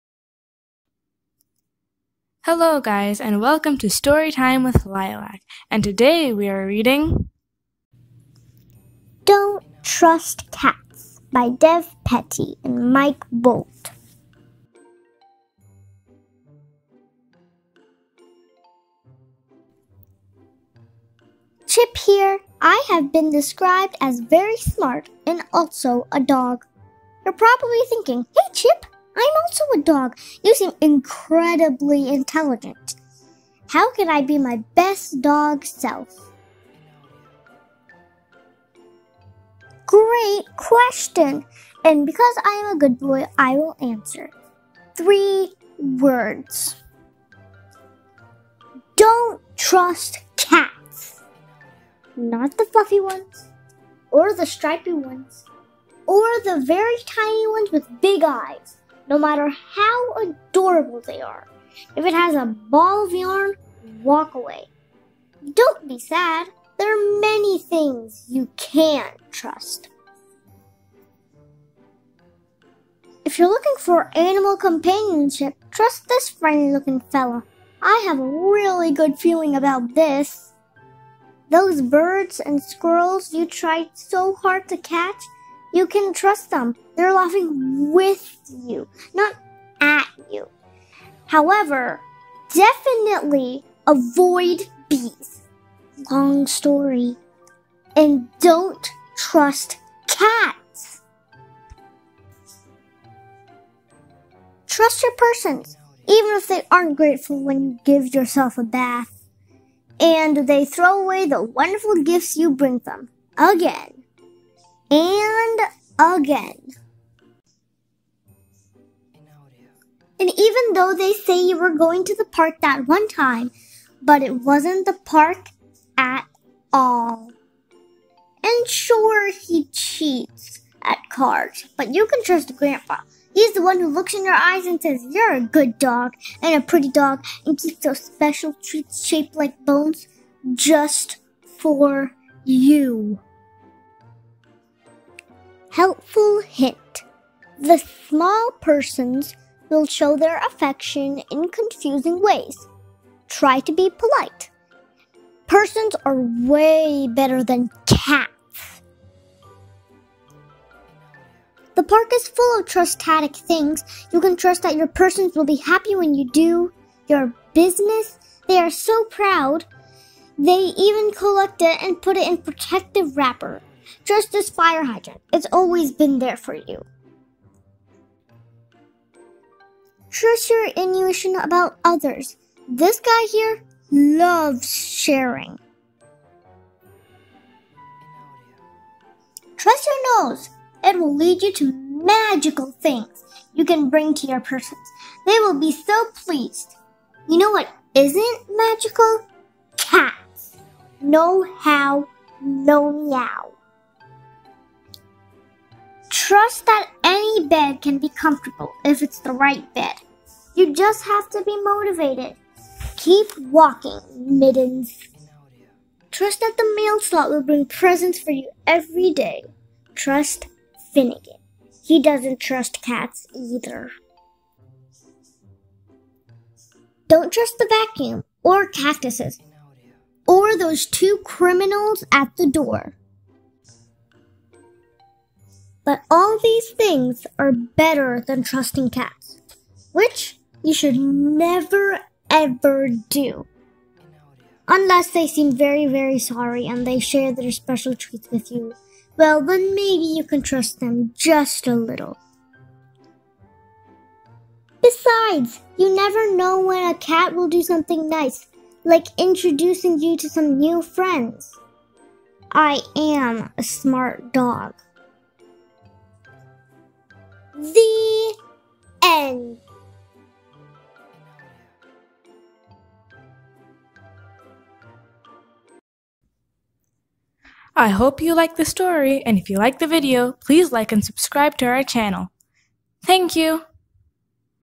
Don't Trust Cats: Life Lessons from Chip the Dog Read Along with Lilac
Don't Trust Cats: Life Lessons from Chip the Dog

Chip the dog from Don't Eat Bees is back, with more "wisdom" to share, including some hard-won life lessons about cats. From the favorite picture book comic duo of Dev Petty and Mike Boldt.

Chip has been described as very smart and also a dog. And because he's a very good boy, he's going to share his tips on how to live your best life, namely: Don't trust cats. Not even the little, fluffy ones with big eyes. Nohow, no meow.

Trust the squirrel you always chase, trust grandpa (sure, he cheats at cards but he always has treats), and trust that skunk (he seems nice. What could possibly go wrong?). Do NOT trust cats, especially not Mittens.

In this fabulously funny pet story, dog AND cat lovers will enjoy seeing their own fur-friends in Chip's earnest yet ridiculous guide to life.